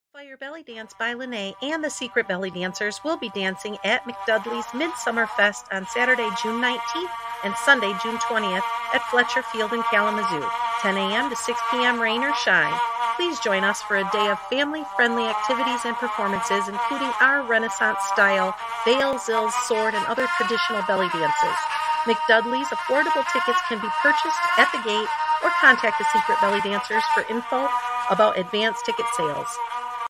The Secret Bellydancers will be at MacDudley's Midsummer Fest 2021!
The Secret Bellydancers will be dancing at MacDudley's Midsummer Fest at Flesher Field in Kalamazoo on Saturday June 19th and Sunday June 20th 2021!